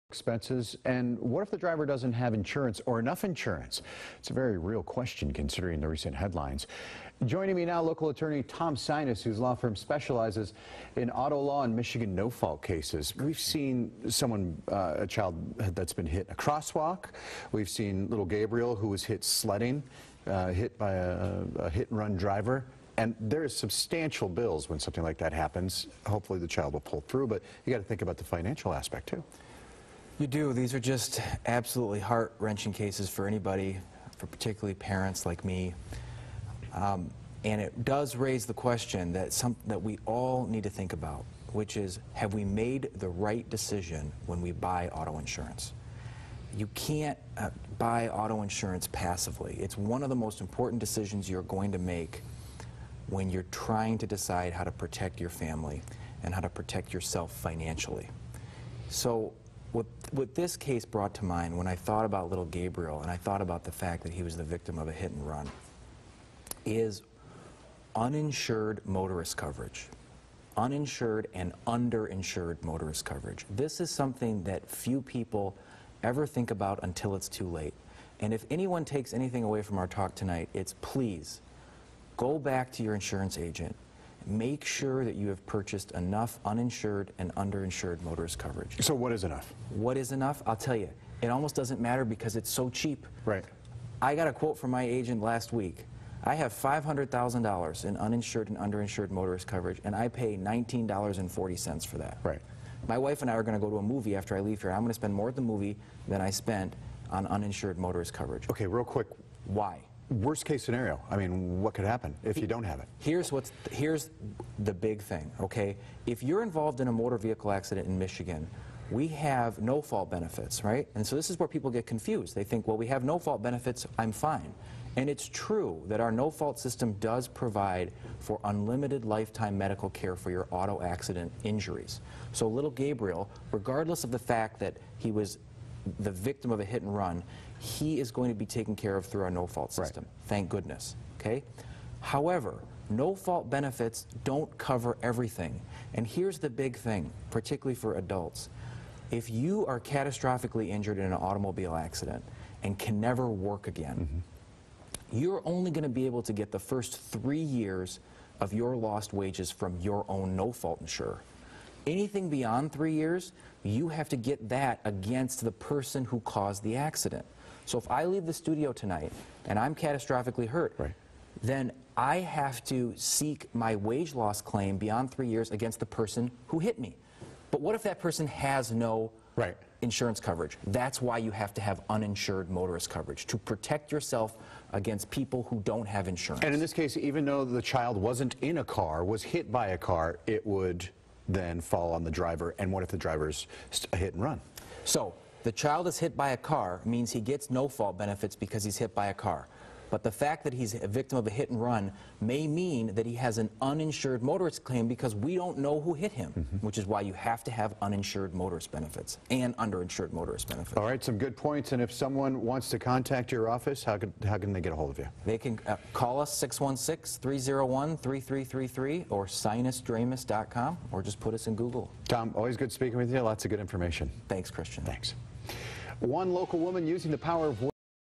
Medical Costs After Michigan Hit-And-Run Accidents
Grand Rapids, Michigan auto accident lawyer Tom Sinas, of the Sinas Dramis Law Firm, discusses the legal issues that arise when a hit and run happens, and how you can protect yourself and your loved ones with the proper insurance coverage.